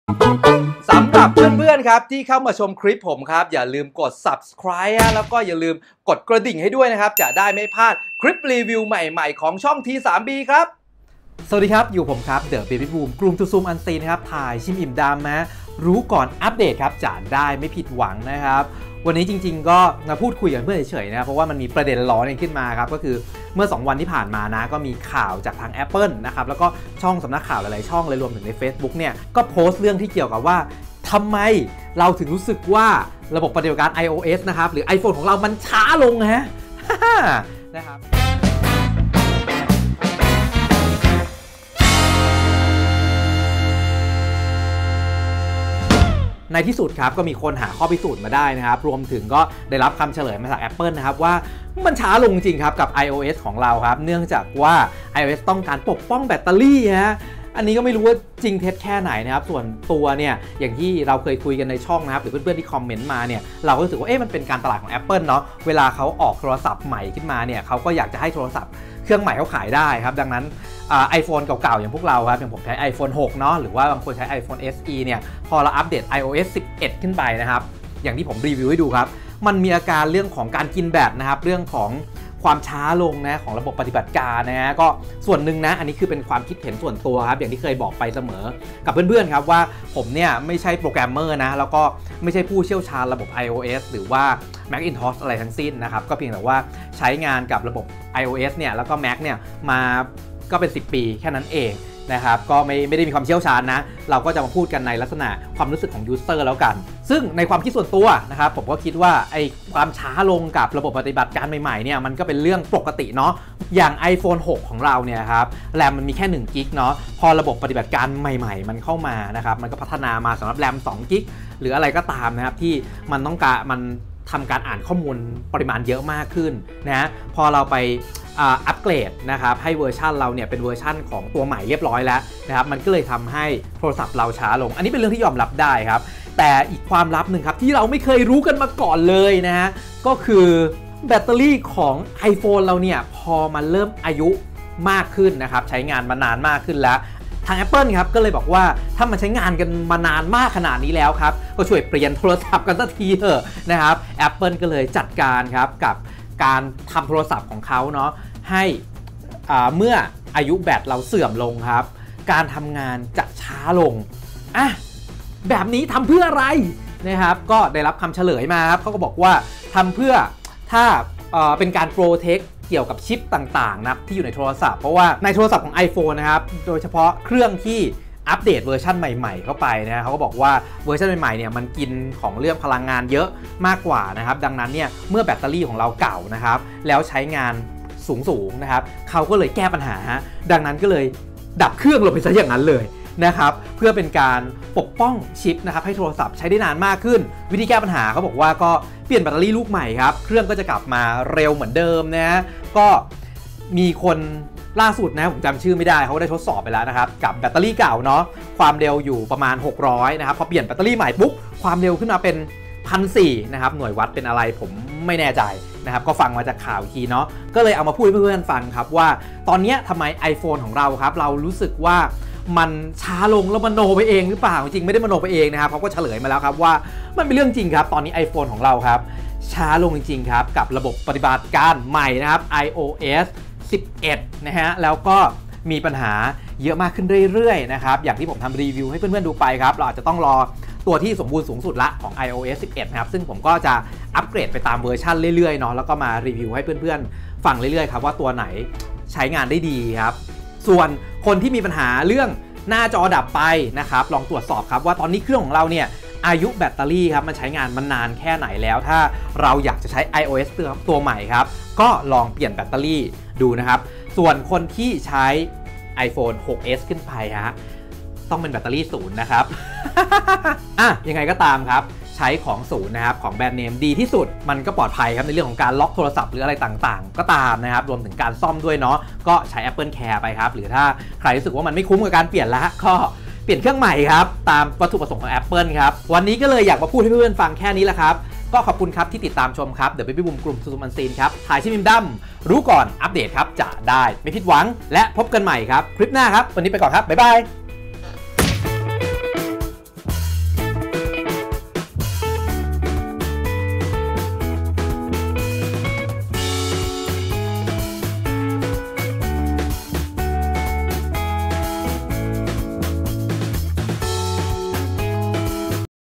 สาเหตุที่ iPhone ช้าลง : T3B
ทำไมเวลา iphone เปิดตัว Model ใหม่เราจะรู้สึกว่าเครื่องเราช้าลงทุกที
ในที่สุด Apple ก็ออกมายอมรับว่า.